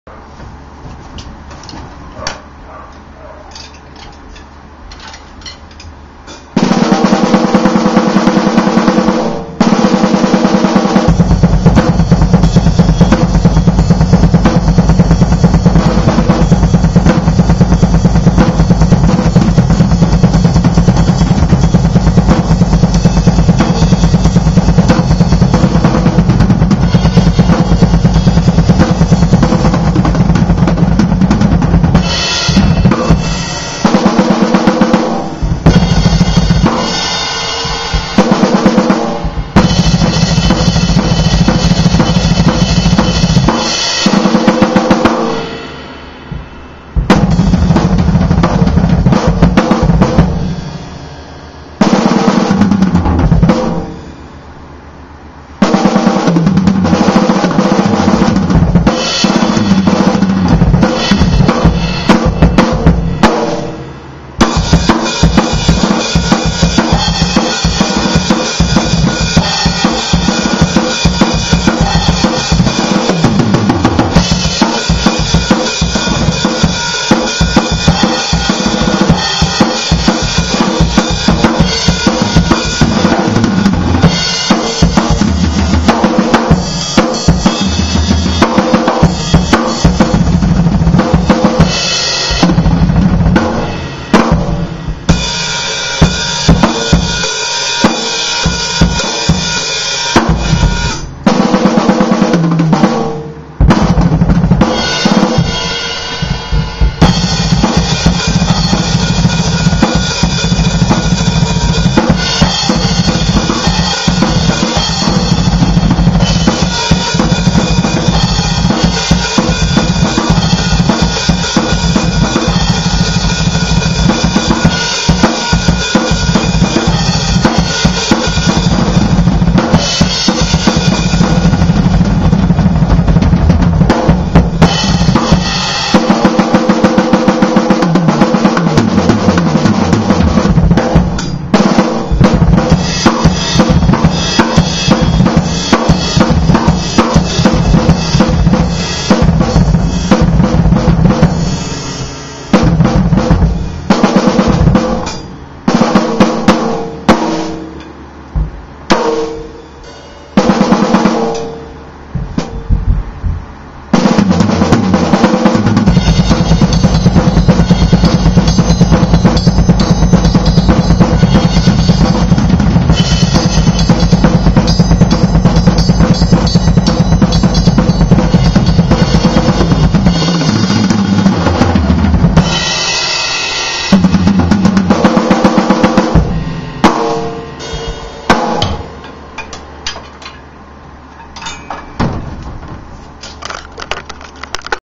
Drum warm up 2 - NHdrummer0832
just playing on the kit trying some new stuff. enjoy!